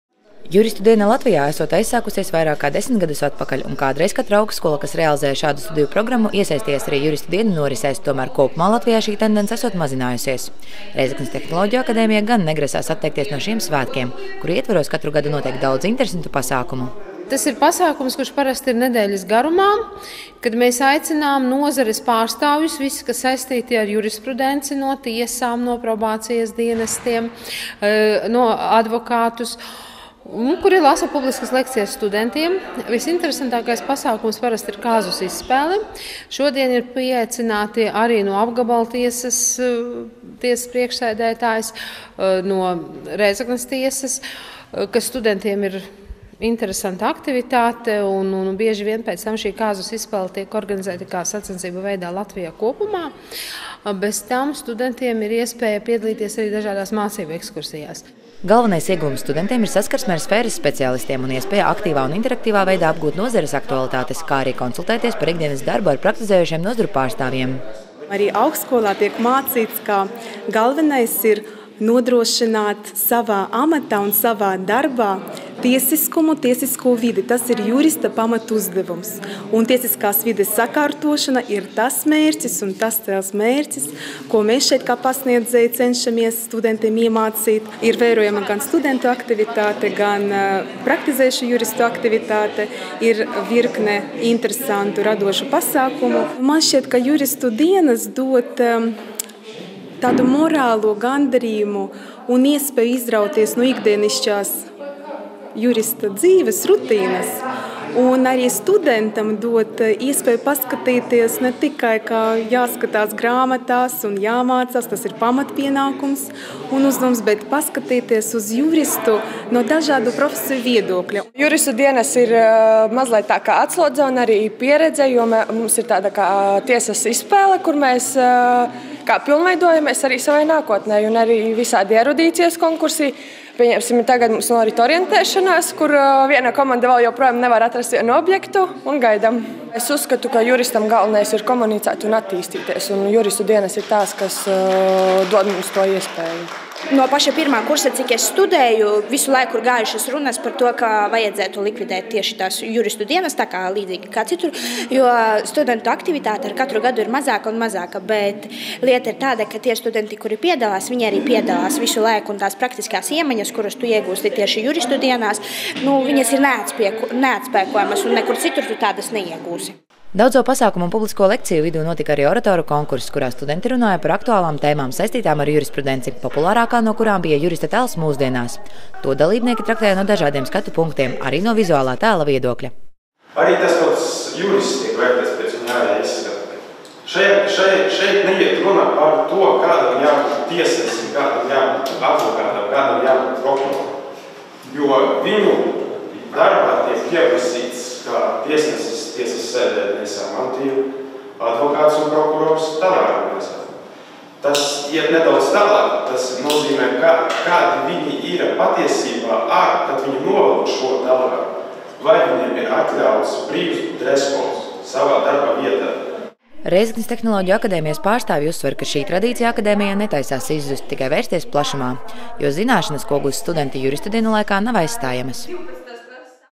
RTA aizvadītas Juristu dienas
Juristu diena Latvijā esot aizsākušās vairāk kā 10 gadus atpakaļ, un kādreiz katra augstskola, kas realizēja šādu studiju programmu, iesaistījās arī juristu dienu norisēs, tomēr kopumā Latvijā šī tendence esot mazinājusies. Rēzeknes tehnoloģiju akadēmija gan negrasās atteikties no šiem svētkiem, kuru ietvaros katru gadu notiek daudz interesantu pasākumu.